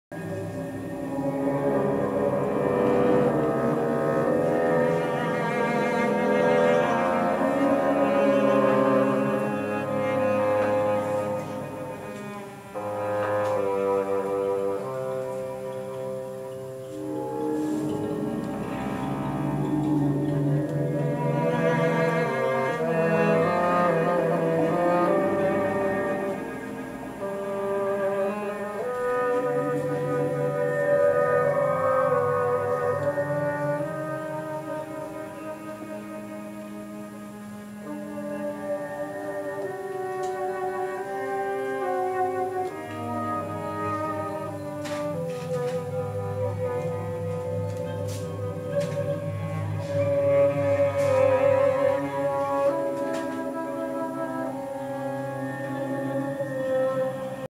Classical Revolution Cincinnati: Camera Lucida part 1
Camera Lucida at Classical Revolution Cincinnati with guest dancers Cherie Ilg Haas and Holly Price and bassoonist Jackie Royce. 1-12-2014 at the North End Tavern.